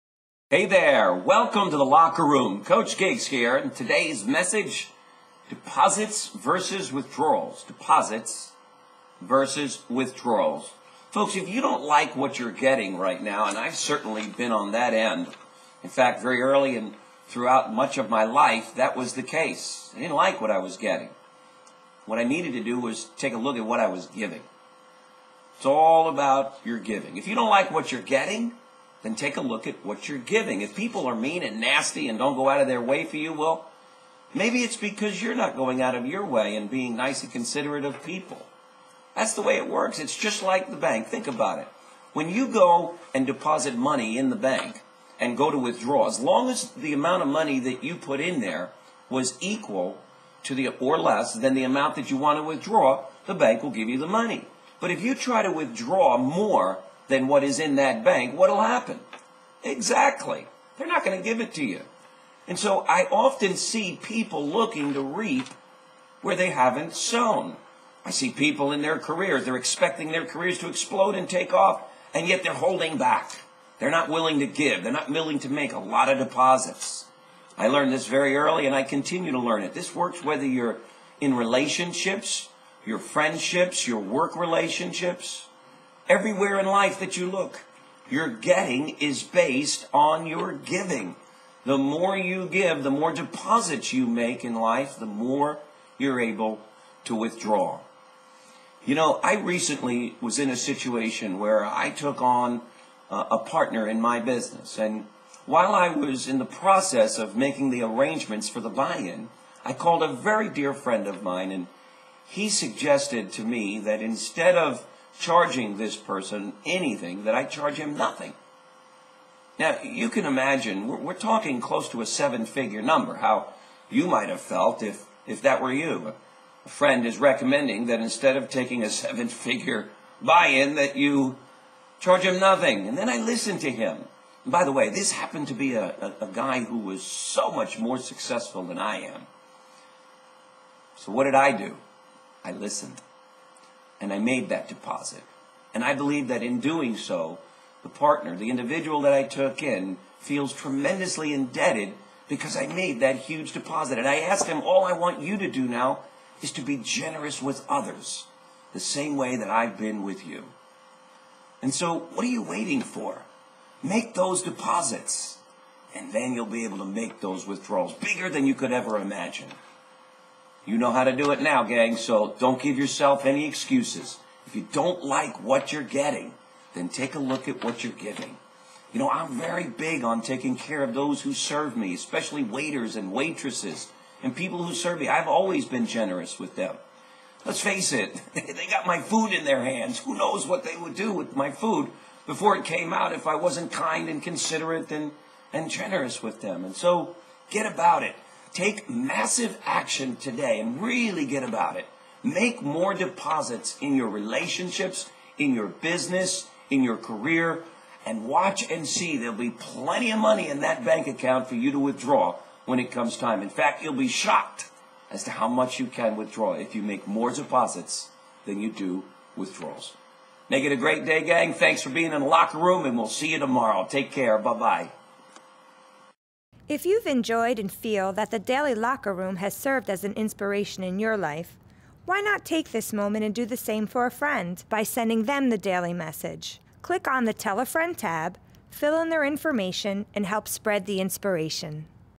Deposits vs Withdrawals - Life Coach Eric Giglione from The Daily Locker Room
If you don't like what you're "getting" from your relationships, career or business, then look at what you're "giving". Give others what they want and need and you'll get what you want and need. The last time you made a bank withdrawal, did you ask for more than you deposited? Of course not, that would be ridiculous.

The same is true in life. Too many people expect back something they have never given. If you want respect, give respect. If you want people to be kind and considerate, you must be kind and considerate. If you want more rewards from your career or business, you must give more and become excellent at your work.

What are you waiting for? Take action today and start making more "deposits". Give more in your relationships and they'll give back to you! Give more at work and you'll earn more and get promoted. Give passionately to the people you serve and your clients will recommend you and brag to everyone they know.

Add value to other people's lives and watch your "bank account" grow beyond your wildest imagination!